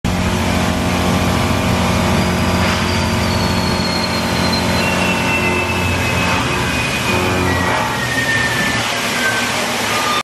Tooling on a Die
CNC machines will accurately make alterations to dies.